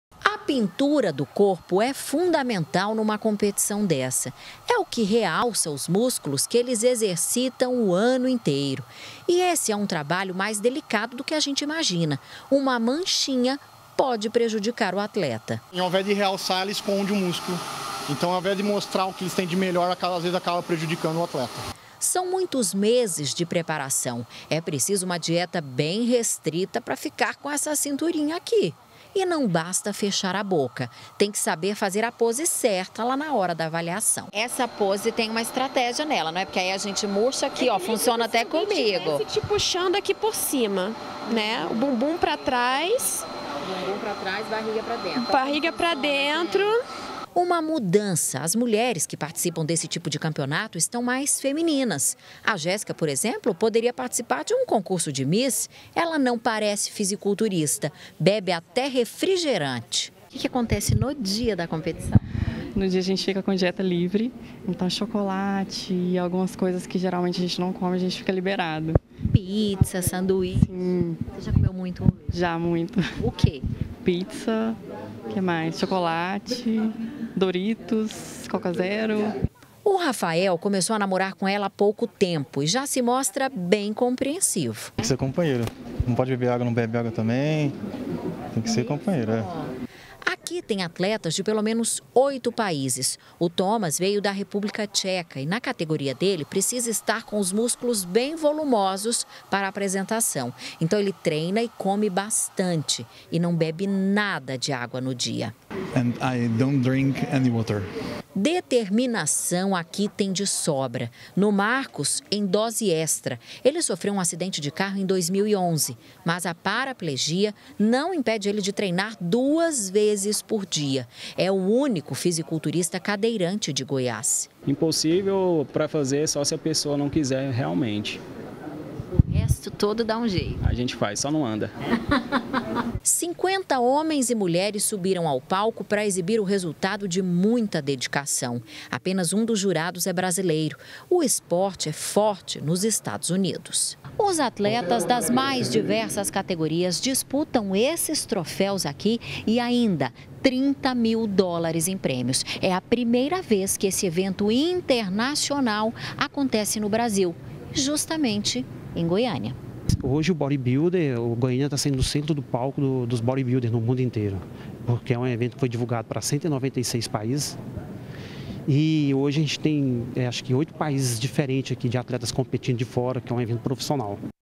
GA - Goiânia sedia evento de fisiculturismo - 12-03-2018
Fisiculturismo. Nesse fim de semana teve evento internacional dos fortões em Goiânia. Pensa num povo forte!. A Fernanda Arcanjo foi ver de pertinho e mostra pra gente agora.